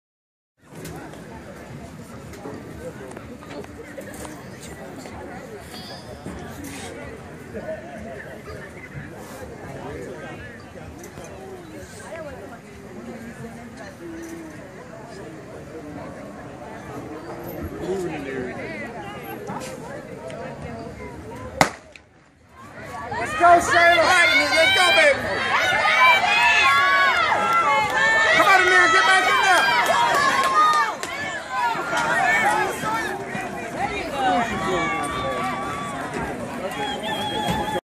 Girls 100 Meters Finals HS 6A Regional Tift Co. GA
April 17, 2014 HS Regional 6A Girls 100 meters final Tift County HS, GA